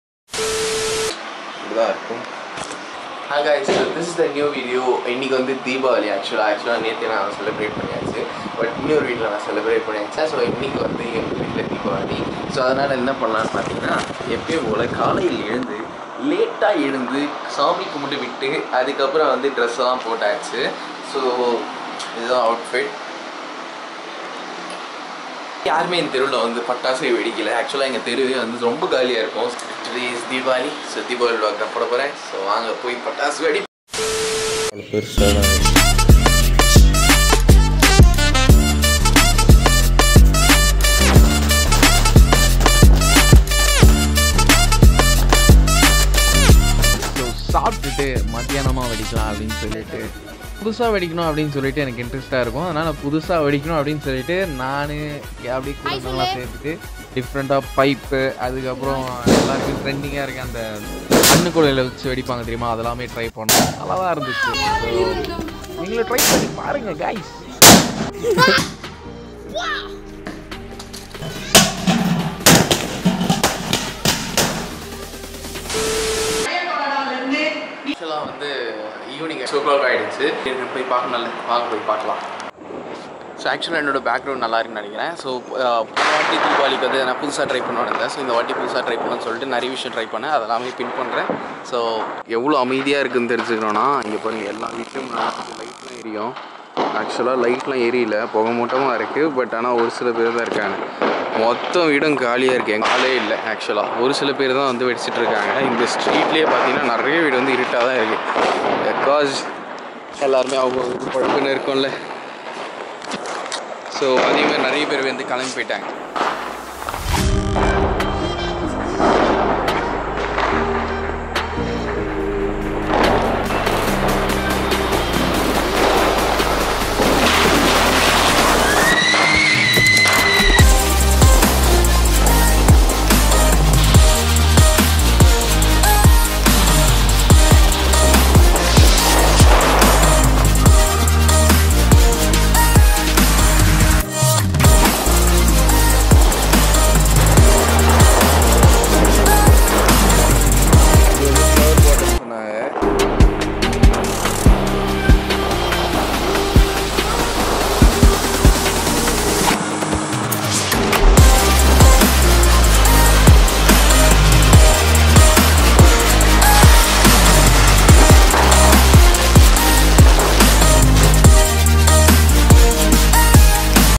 DIWALI VLOG | இனிய தீபாவளி நல்வாழ்த்துக்கள் | Vivek Tamil Traveler
00:00 Introduction
00:34 Crackers
00:52 New try with crackers
01:18 Evening crackers gone wrong
01:59 sky shots cinematic
02:38 Ends subscribe